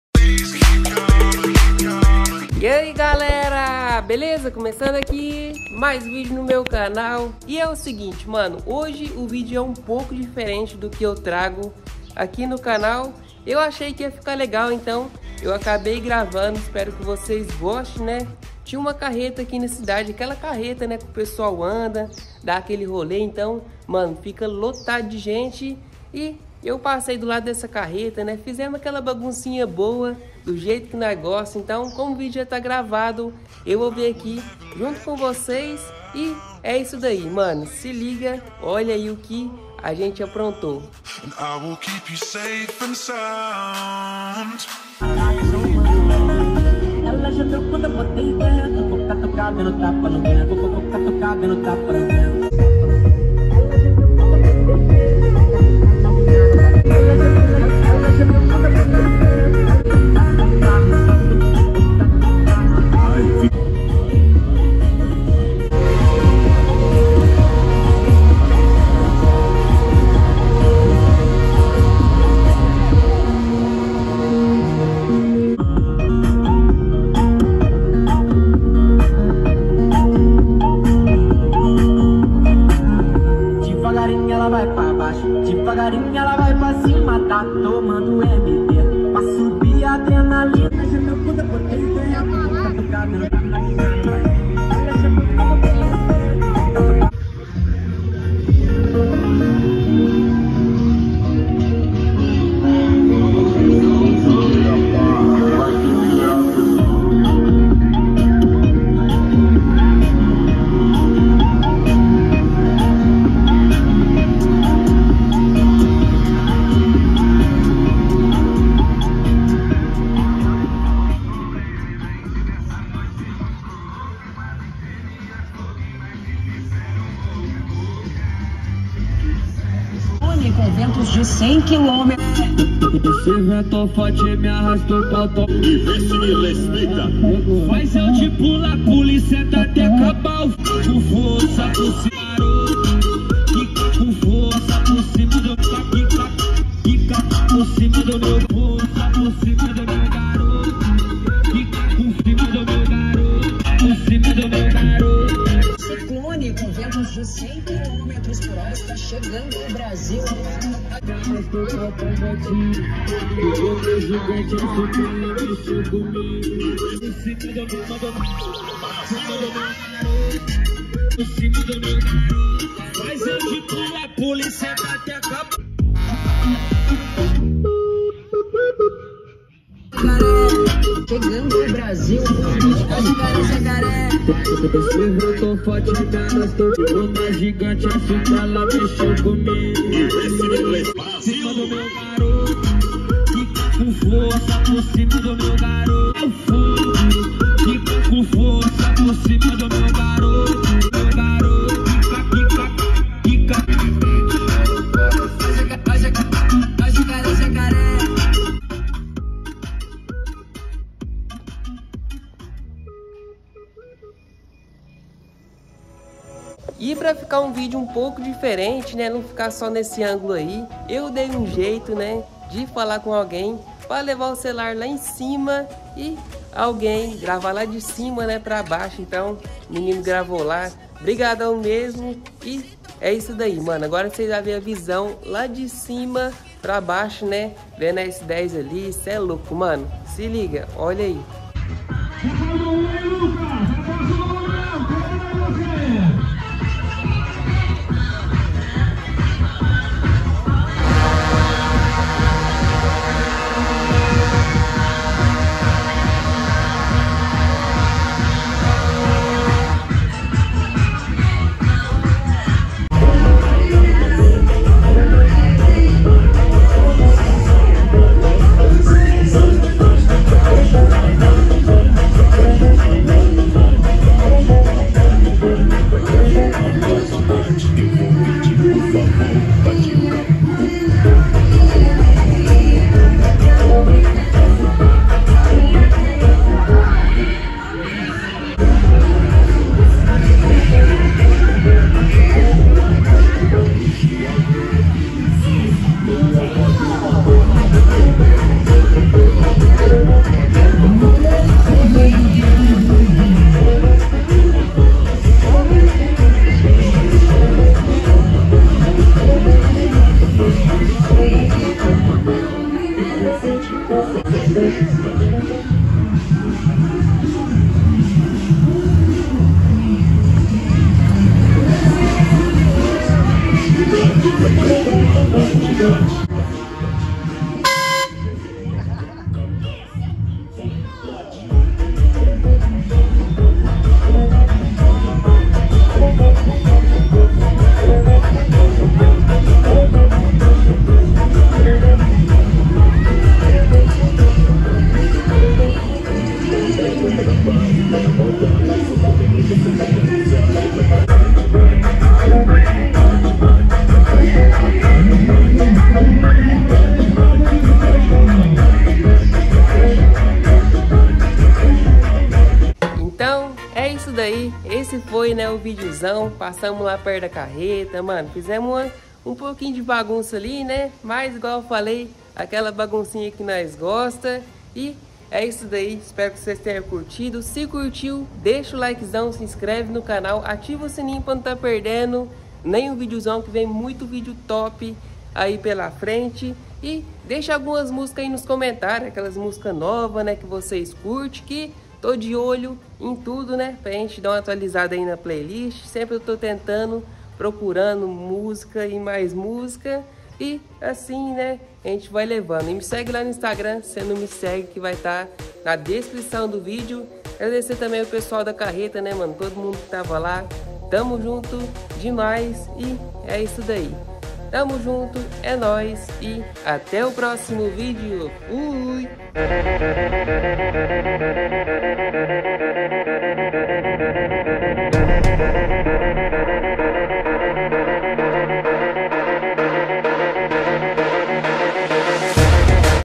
ENCONTREI UM FLUXO AMBULANTE S10 CAUSANDO 😈
Eae galera beleza nesse vídeo passão ao lado da carreta lotado de gente espero que vcs tenham gostado do vídeo se gostou deixe seu like se inscreva no canal e ative o sino de notificação

TMJT GALERA